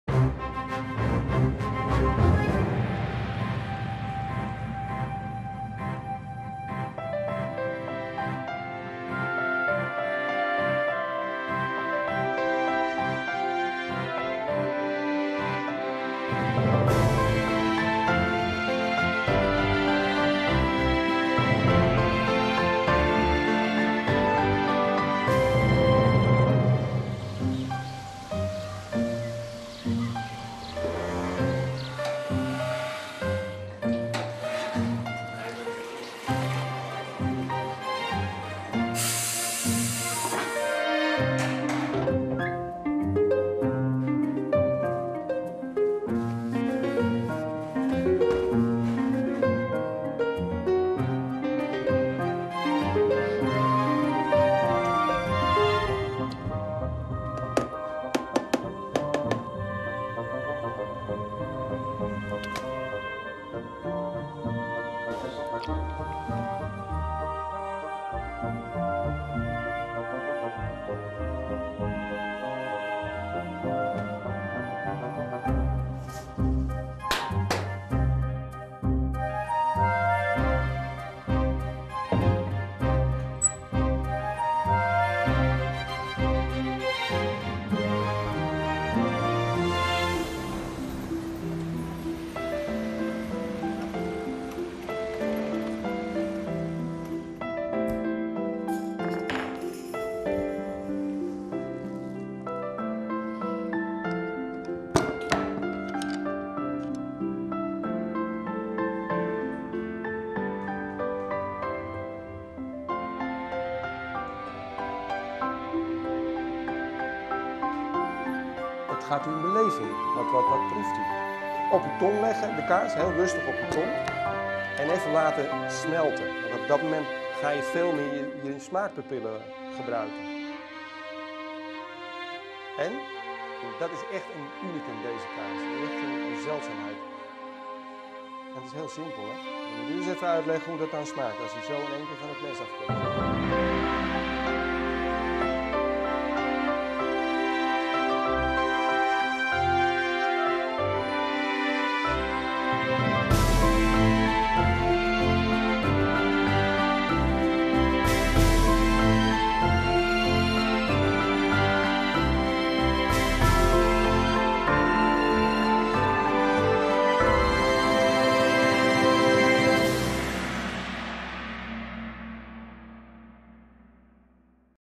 Reypenaer Kaas - Nederlands
In het 100 jaar oude kaaspakhuis aan de Oude Rijn in Woerden rijpen de kazen zoals vroeger. In het Reypenaer Proeflokaal aan de Singel 182, in Amsterdam kunt u zelf de smaak, de geur en de hoedanigheden proeven.